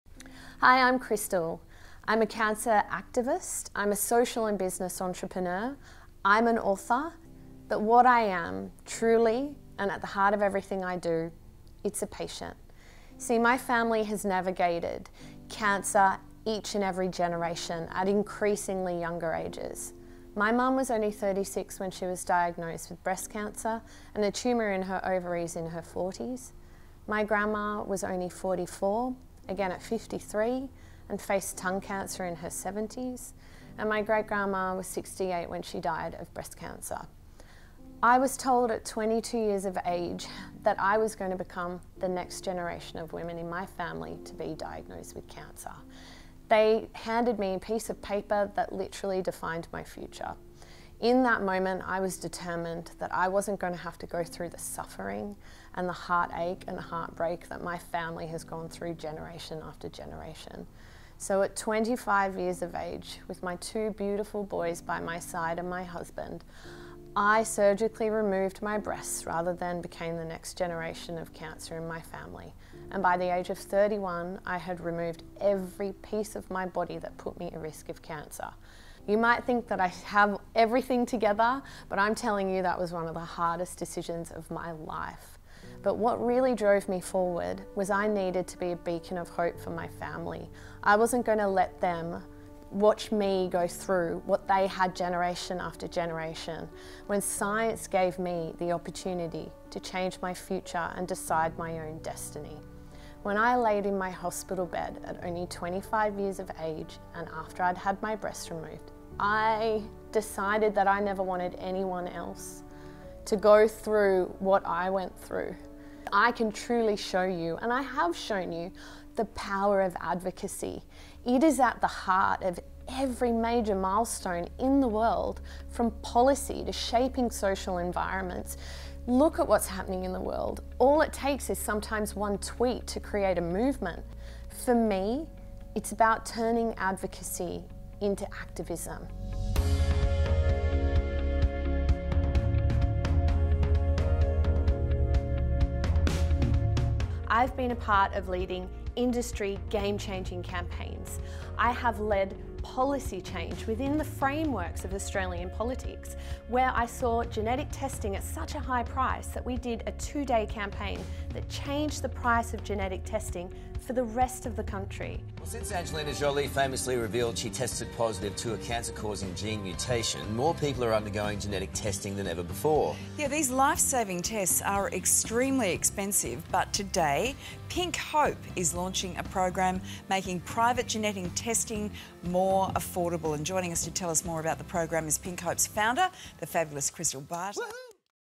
Our Founder's Story
Meet Krystal Barter, the Founder and CEO of Humanise Health.

Her story is born out of the sheer determination to break the cycle of cancer that has plague her family for generations. Armed with knowledge that she was in fact a carrier of a cancer-causing genetic mutation, BRCA1, she decided to change her future and take control of her health. At 25, Krystal surgically removed her breasts rather than become the next generation of cancer in her family.

But the story didn’t end there. Krystal created a movement called Pink Hope, focused on ensuring other families like hers had answers, knowledge and options to determine and mitigate their own cancer risk. In over 10 years, Krystal and her team has grown Pink Hope into a force for change and a household name.

A strategic and solutions based-entrepreneur, Krystal has never been one to take a traditional approach to problems. Through Humanise Health, she is now revolutionising the healthcare industry, alongside businesses, by creating new patient-led, patient driven strategies and projects to ensure optimal outcomes for consumers and business alike.

“It’s time for change, to optimise consumer trust and health outcomes, and to truly humanise health.” – Krystal Barter